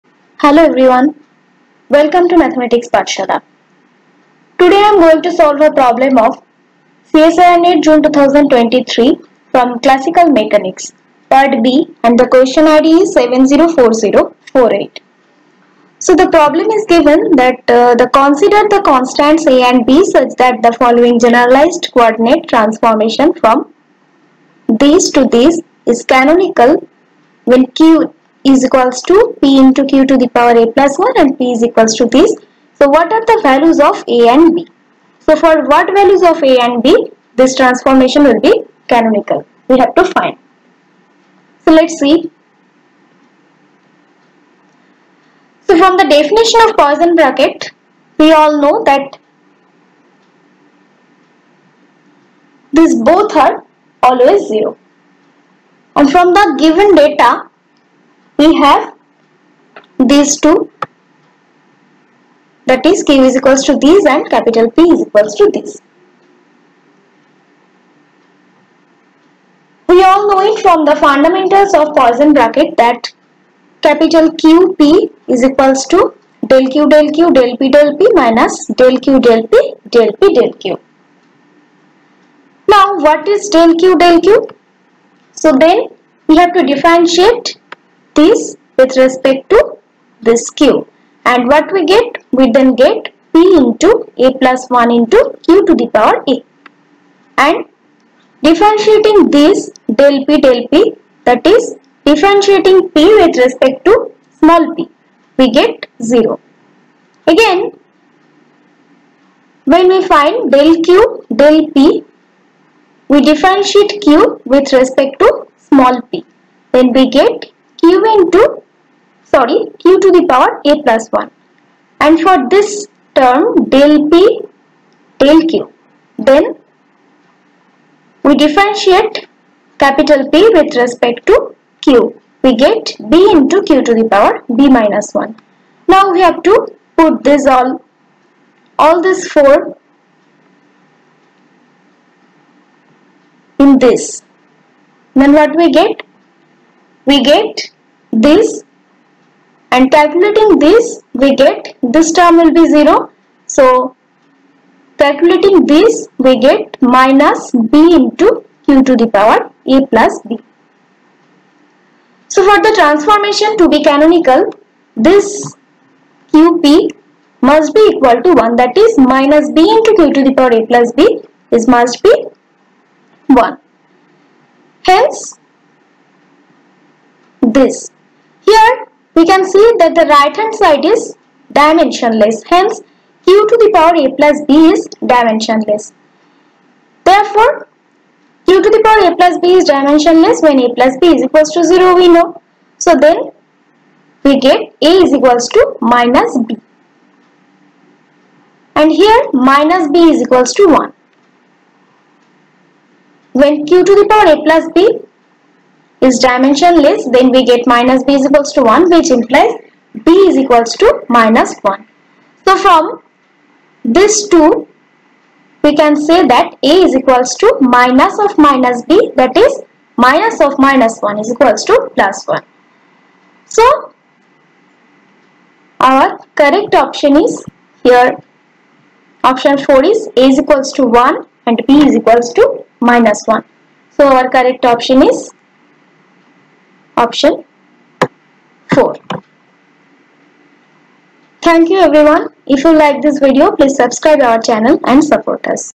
CSIR UGC NET June 2023|Mathematical Sciences |Classical Mechanics | Part B | Question ID 704048
Classical Mechanics
Mathematical Science
Part-B Question, CSIR NET June 2023 Exam.
Question ID 704048:
Consider the constants a and b such that the following generalized coordinate transformation from (p,q) to (P,Q) is canonical
$Q= p{q^(a+1)} , P=q^b$
What are the values of a and b ?
(1) a= -1, b= 0.
(2) a= -1, b= 1.
(3) a= 1, b= 0.
(4) a= 1, b= -1.

We have found the correct solution. Option (4) is correct.